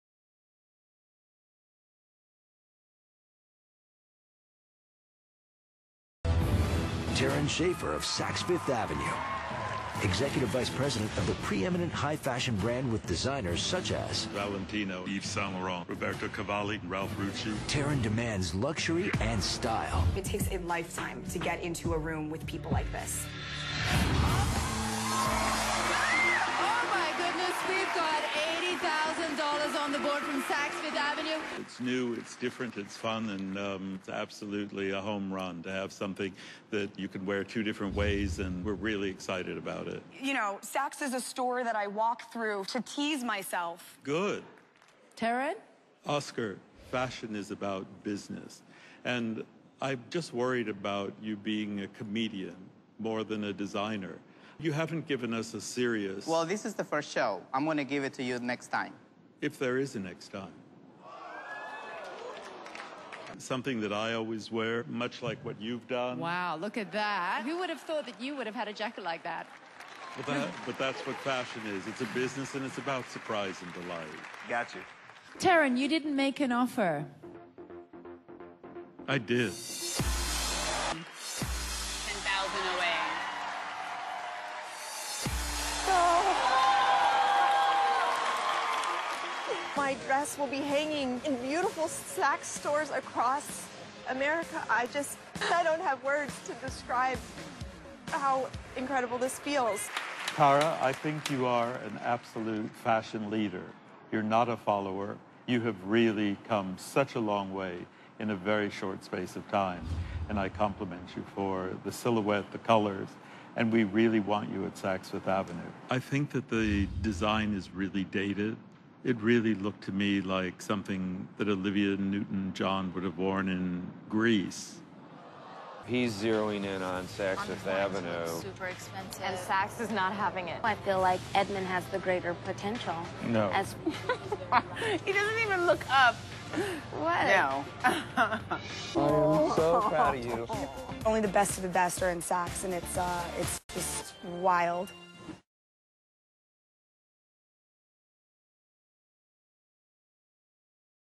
Terron Schaefer is Guest for 2013 Passion for Fashion
Video of Terrron Schaefer of Saks Fifth Avenue and Fashion Starrt